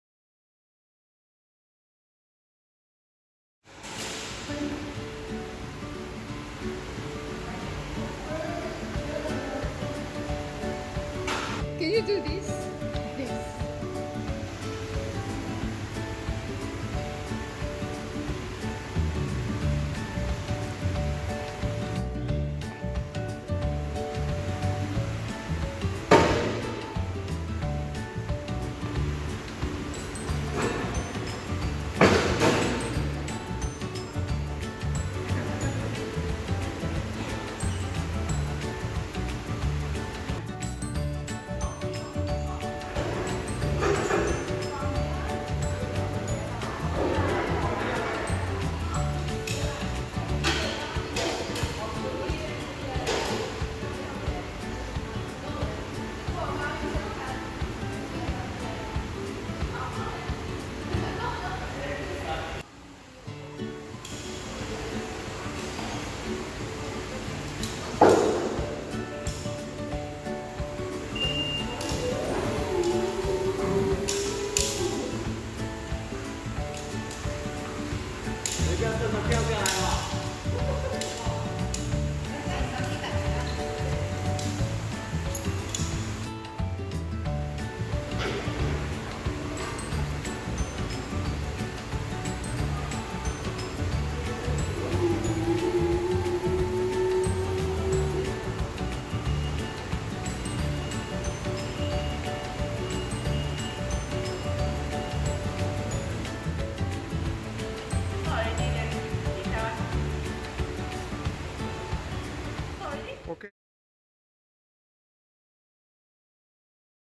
Prevention as the cornerstone for safety 2 · Haulotte Singapore
Since the very beginning of Haulotte, safety has been the cornerstone of our actions and of our products. Safety is vital when working at height solutions.

Haulotte SEA, as part of their Townhall activity divided their teams into groups and given 90mins to produce a safety video with their mobile phones. We hope everyone think safe, work safe and get to go home well at the end of the day.